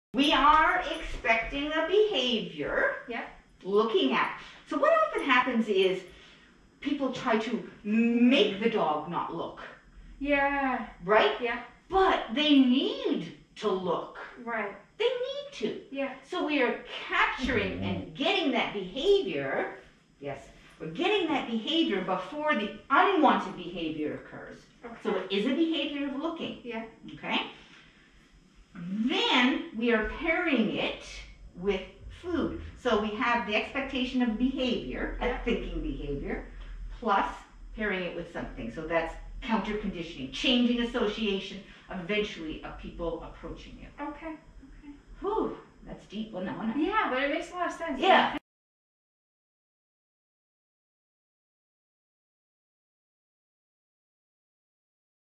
A little Dog Training Nerdy talk
A little science why we might reward our dogs for looking at a potential trigger. Rewarding the look before the bark/lunge /bite is an alternative thinking behavior  PLUS if we pair it with a yummy food treat we are changing associations .  However we do need the appropriate distance away